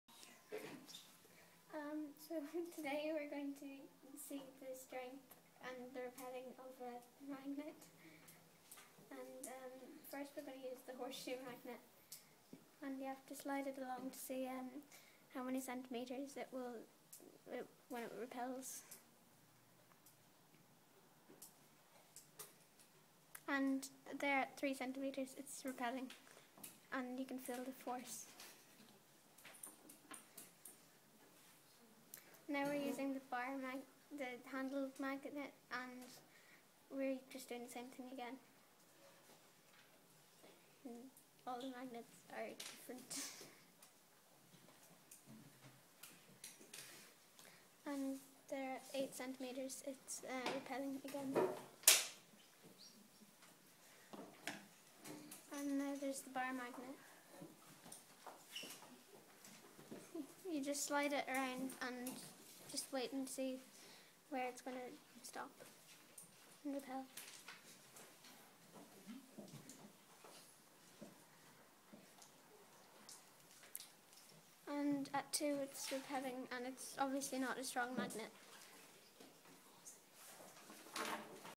Testing the strength of a magnet ( Sarah & Jessica)
Sarah and Jessica demonstrate how to test the strength of a magnet using a ruler, blu-tack and magnets.